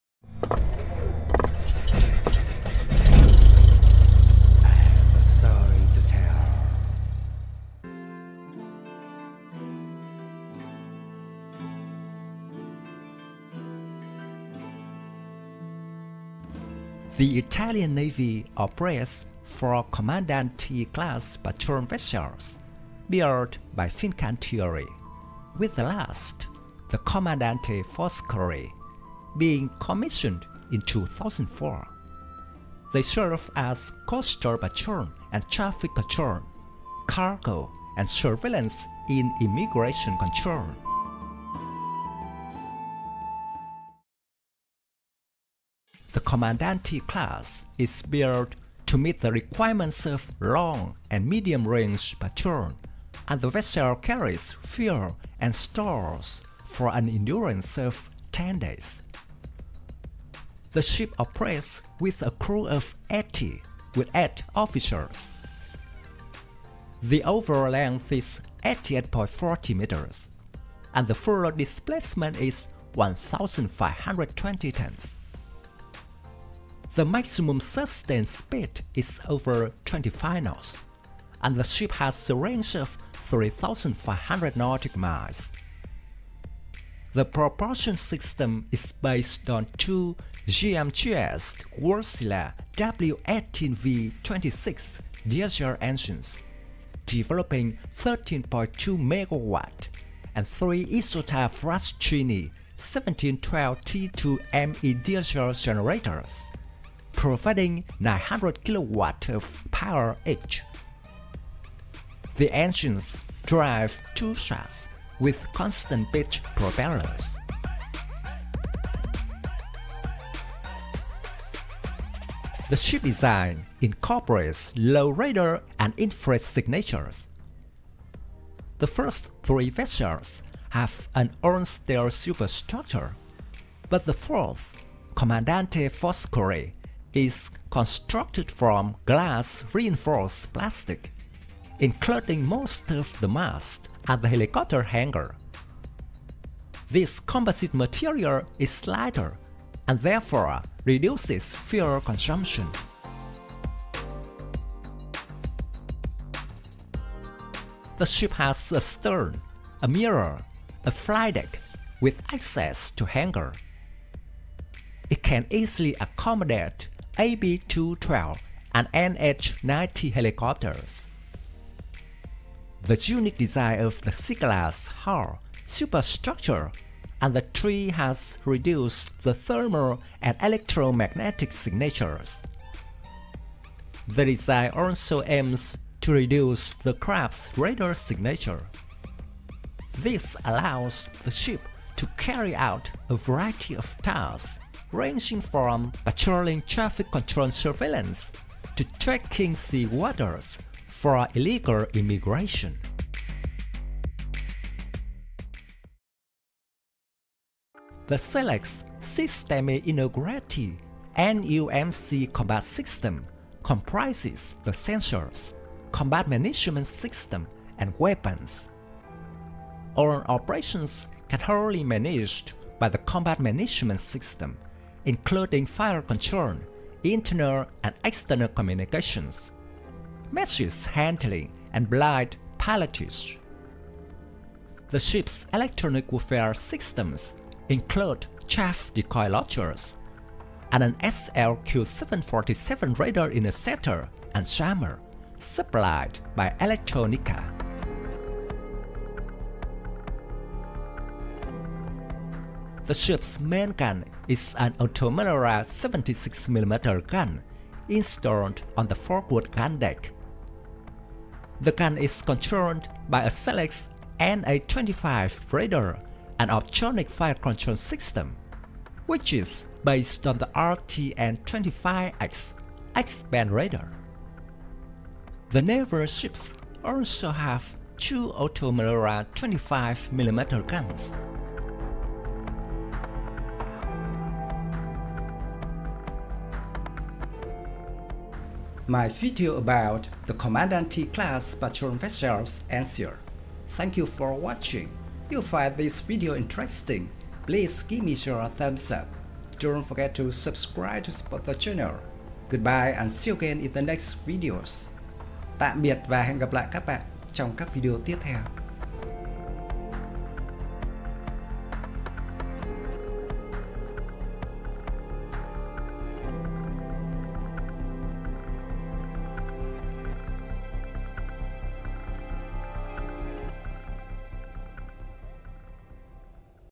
Comandanti-class: Main Class of Patrol Vessels of the Italian Navy
The Italian Navy operates four Comandanti-class patrol vessels, built by Fincantieri, with the last, the Commandante Foscari, being commissioned in 2004. They serve as coastal patrol and traffic control, cargo and surveillance in immigration control.
The Comandanti-class is built to meet the requirements of long- and medium-range patrol and the vessel carries fuel and stores for an endurance of ten days. The ship operates with a crew of 80 with eight officers.

Thank you very much.
Best regards
Dung Tran.